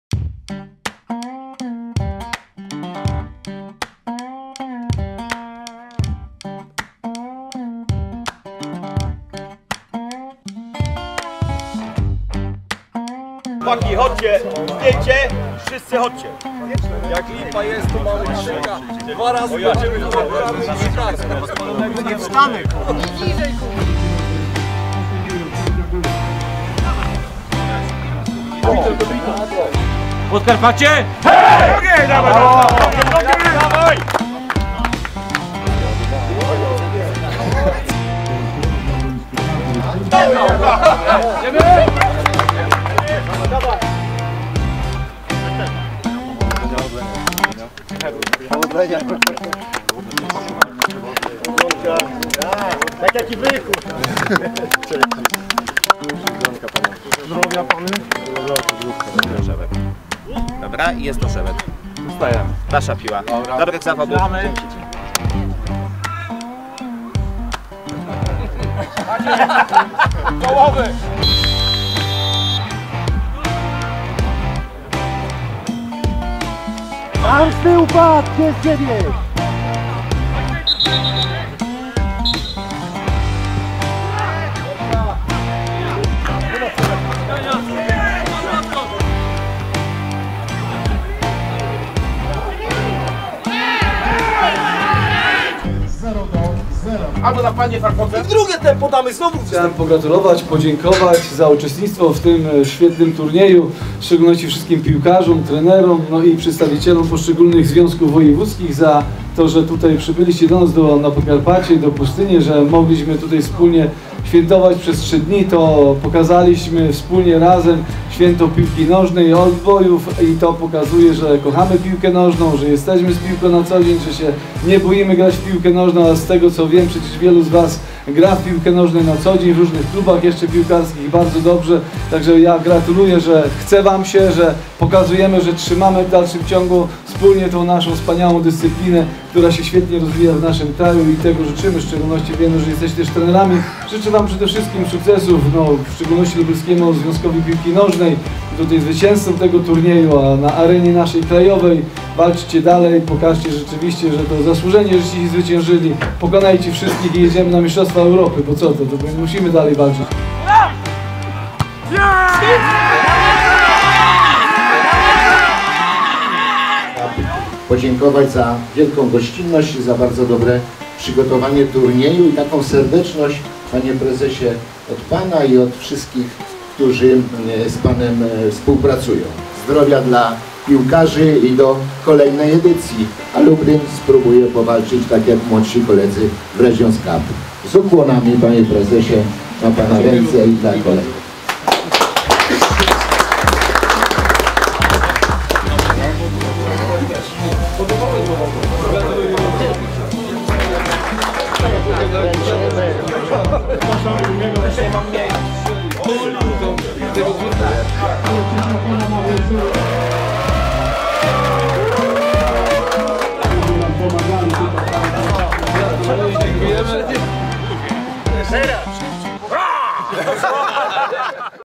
Trzeci i ostatni dzień Turnieju Reprezentacji Oldboy Wojewódzkich Związków Piłki Nożnej
W niedzielę 11 września, byliśmy z kamerą na ostatnim dniu Turnieju Wojewódzkich Związków Piłki Nożnej dla Oldboyów! W decydującym o awansie meczu, Lubelski ZPN wyszarpał "zwycięski remis" naszej drużynie.